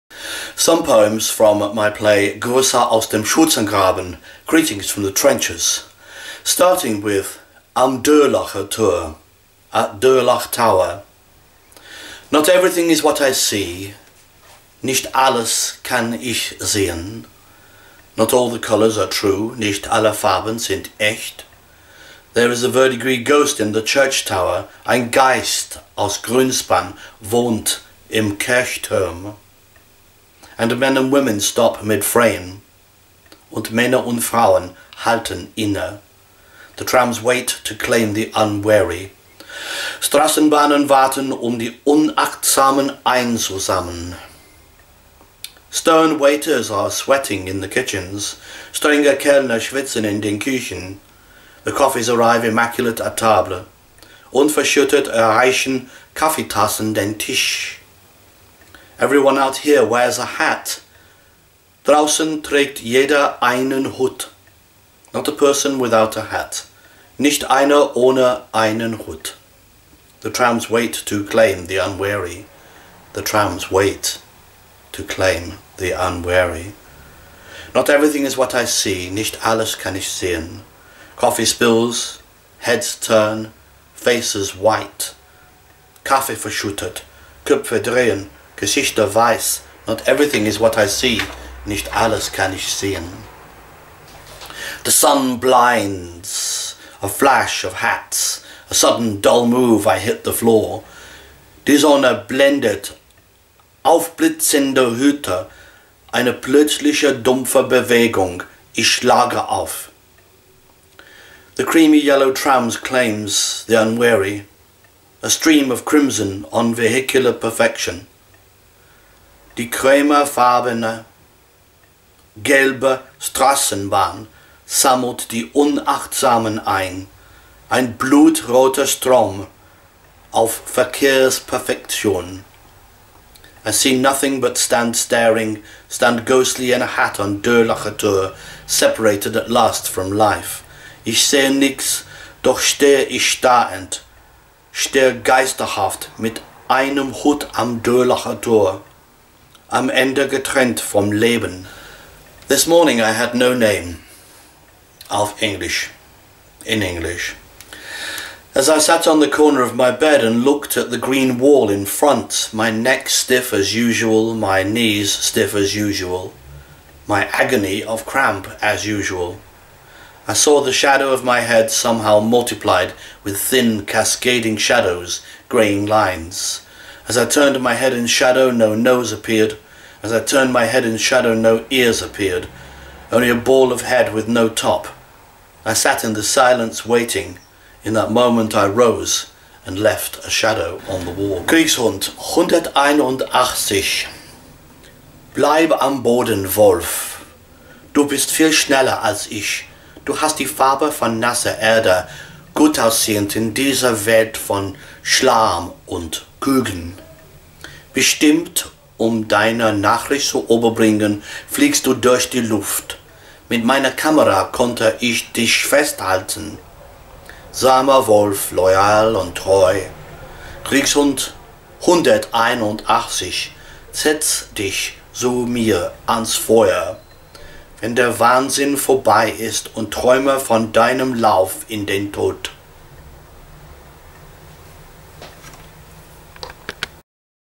Poems in English and German from Greetings From The Trenches.
Poems written and recited by Phil Lowe in English and German from Greetings From The Trenches.

Poems in German were translated by Thorsten Feldmann.